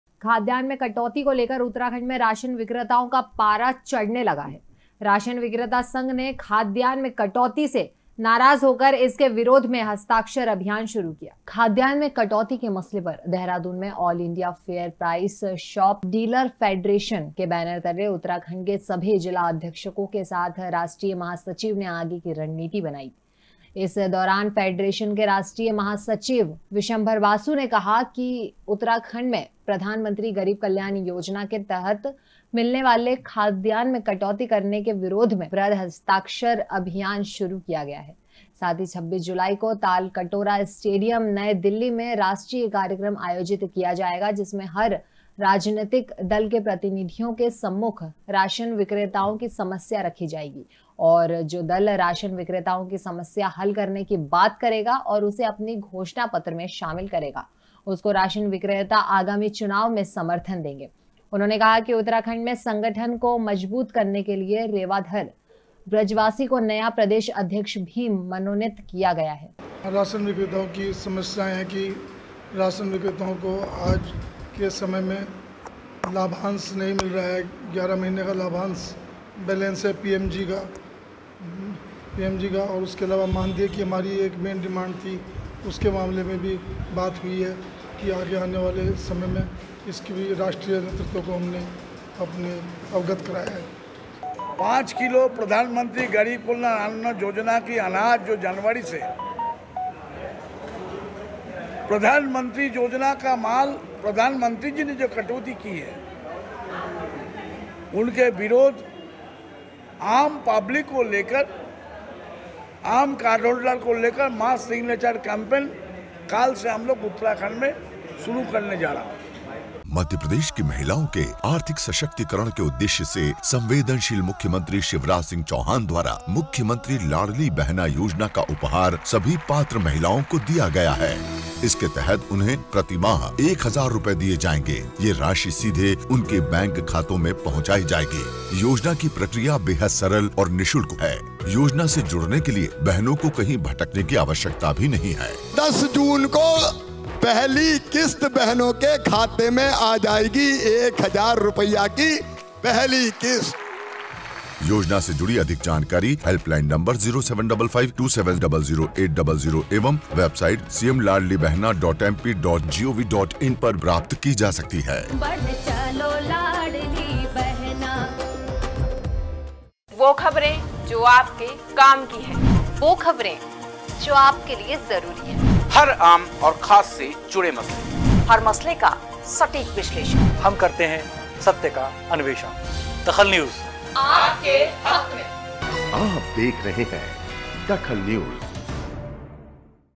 DAKHAL NEWS खाद्यान्न में कटौती के विरोध में हस्ताक्षर अभियान
खाद्यान्न में कटौती के विरोध में हस्ताक्षर अभियान
राशन विक्रेता संघ खाद्यान्न में कटौती से नाराज